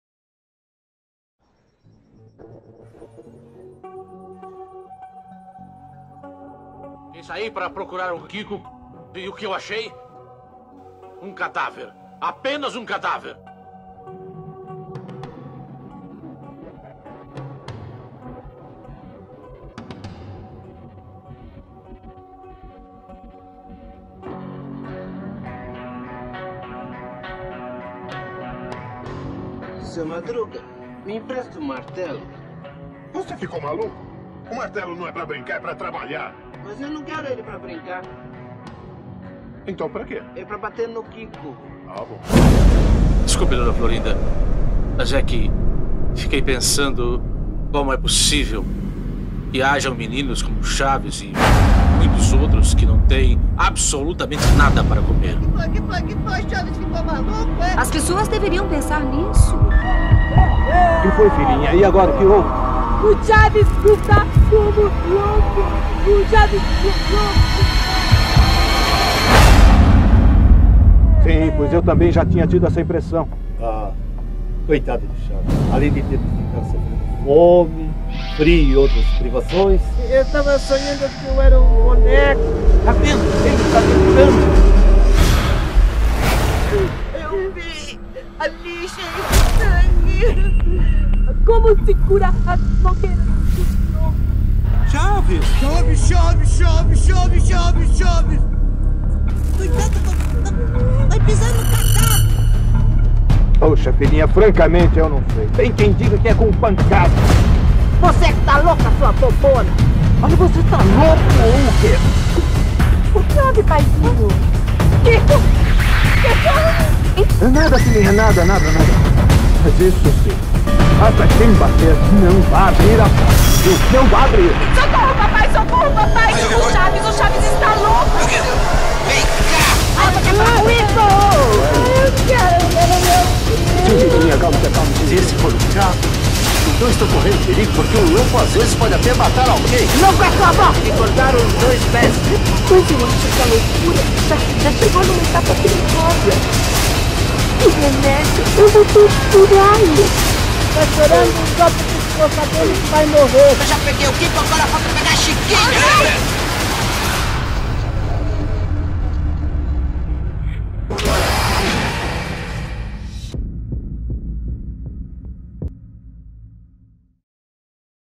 Chaves - Estado de loucura | Trailer
Sinopse: A história de um menino abandonado a própria sorte, vivendo à margem da sociedade, flagelado pela solidão, o frio e a fome, até sucumbir e mergulhar no abismo da loucura. Chaves agora é uma ameaça aos moradores da vila e tem como objetivo "colecionar" seus amados vizinhos dentro do seu barril.

📎 Produção independente: Uma paródia/homenagem para fãs da série.
📎 Doações: Se você apoia o trabalho deste canal e quiser ajudar, aqui está o meu perfil no Paypal para doações: PAYPAL